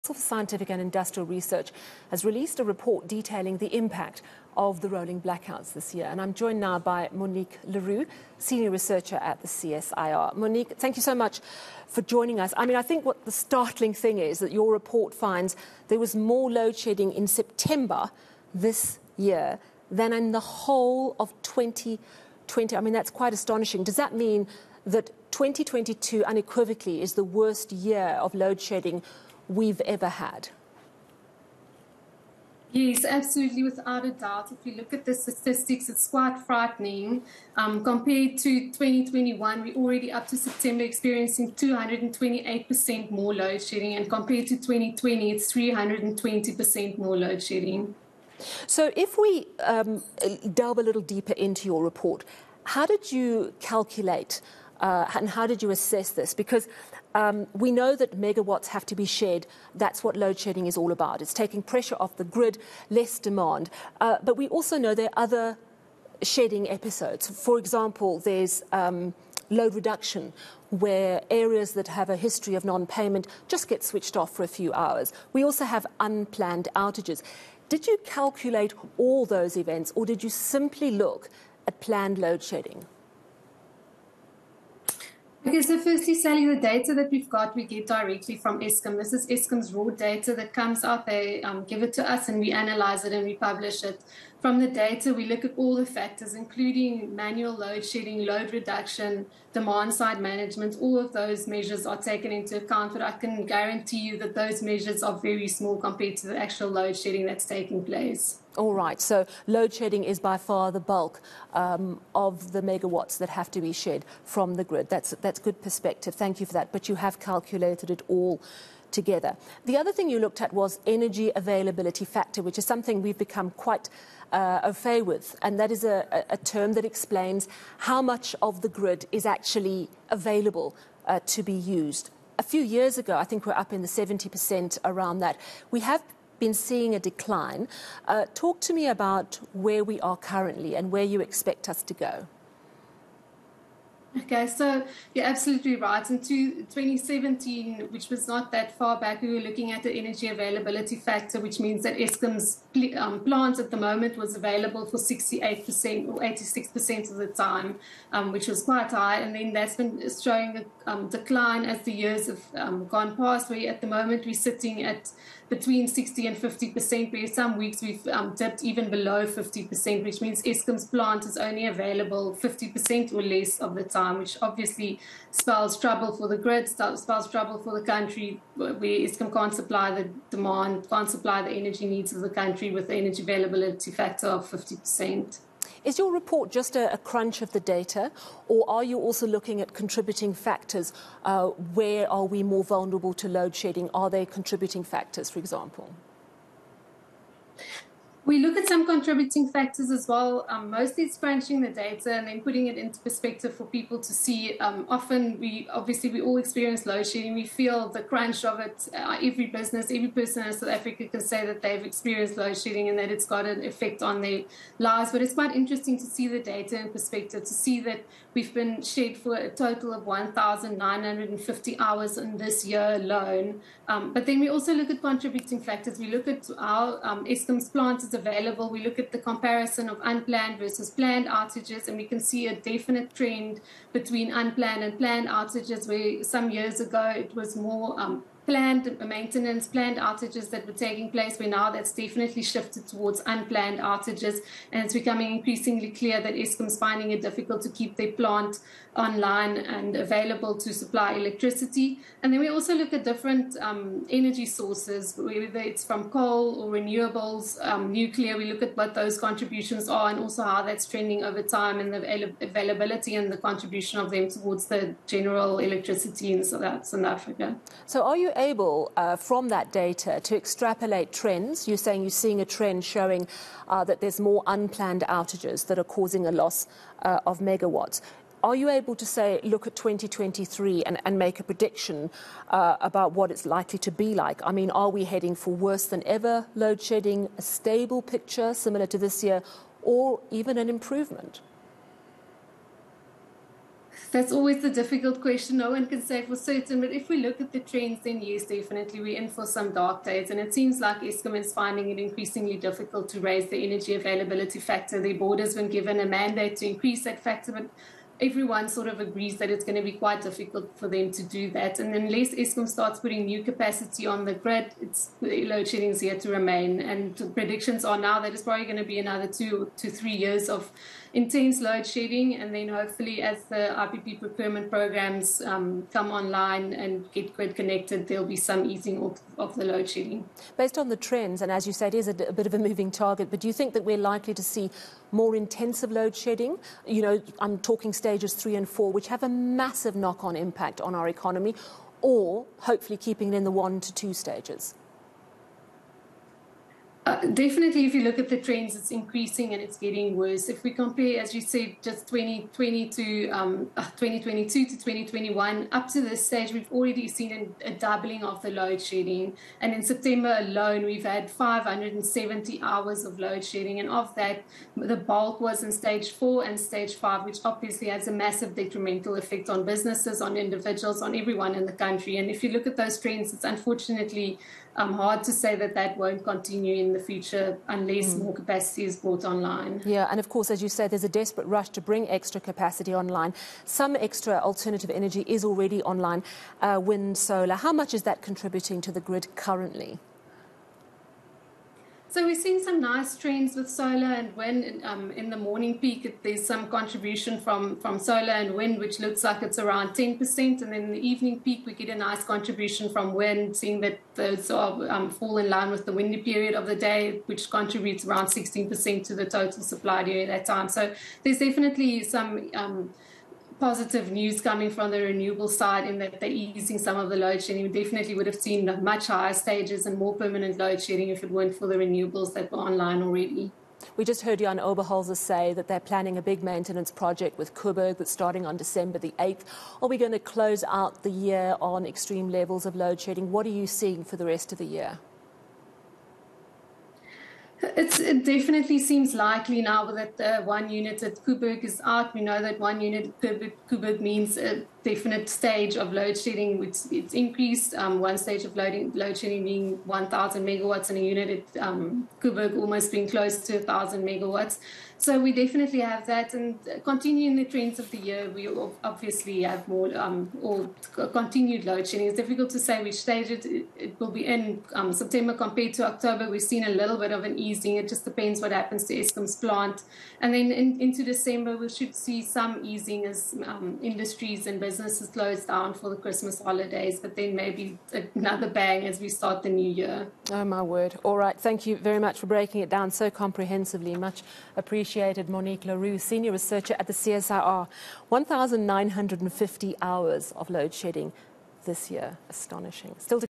CSIR releases blackouts impact report
The Council for Scientific and Industrial Research has released a report detailing the impact of the rolling blackouts this year. For more, eNCA speaks to Monique le Roux, Senior Researcher at the CSIR.